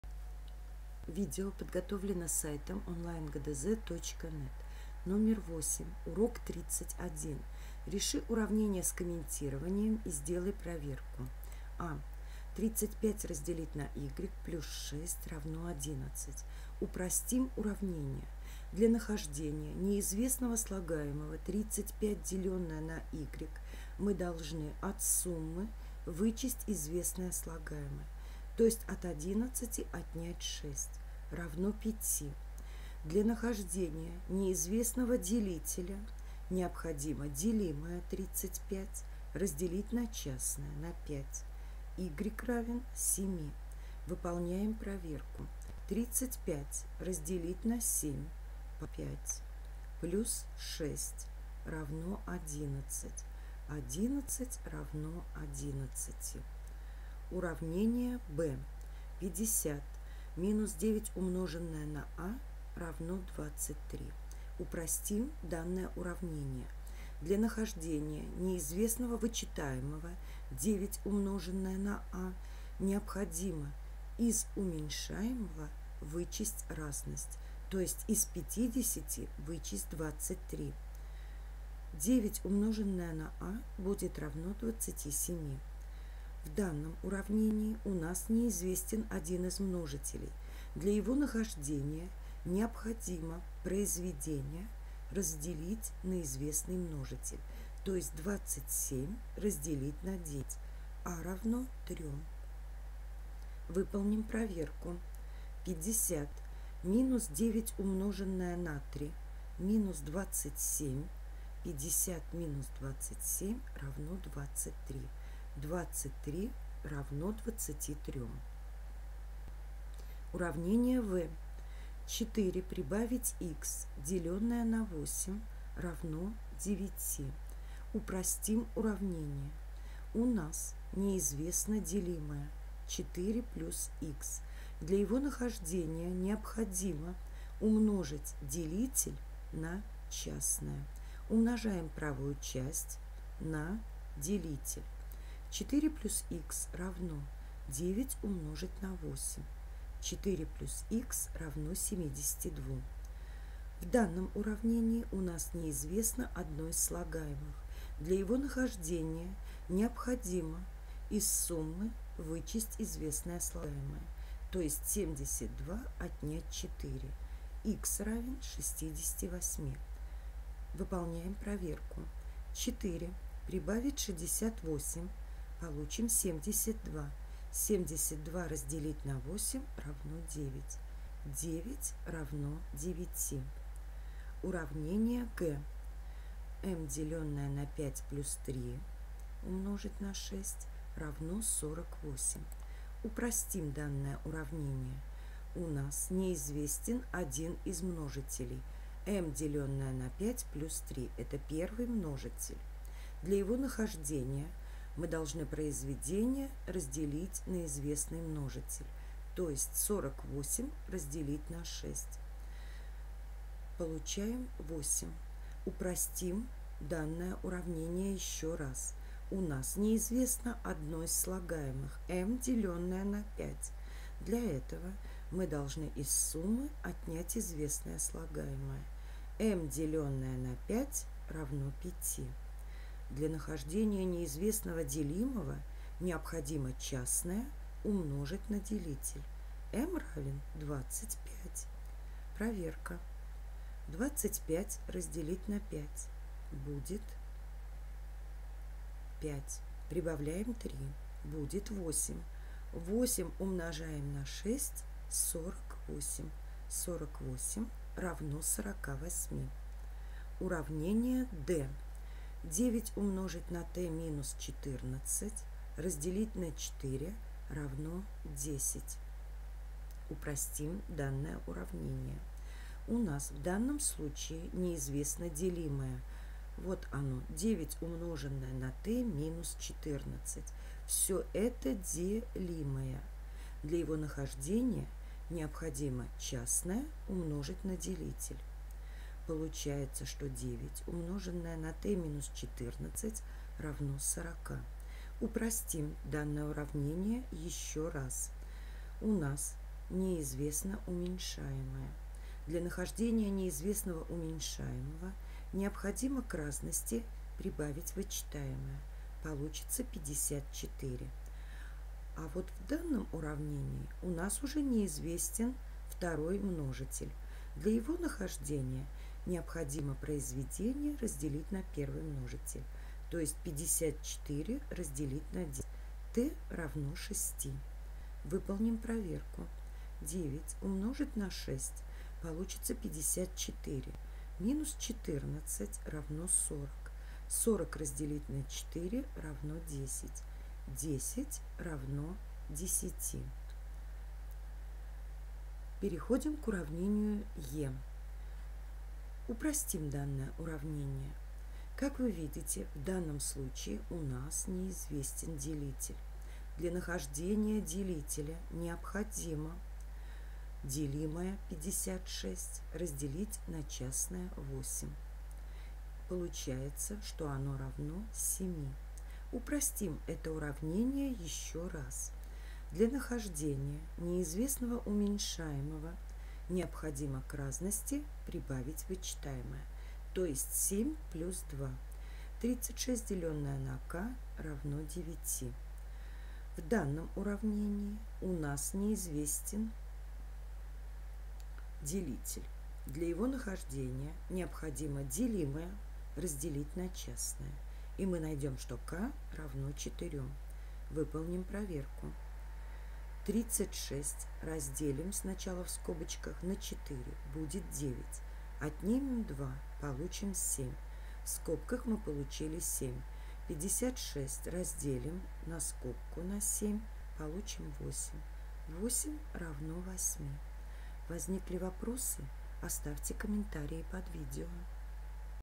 Урок 31 Задание 8 – ГДЗ по математике 3 класс (Петерсон Л.Г.) Часть 2
В данном номере решаем уравнения с комментированием и делаем проверку.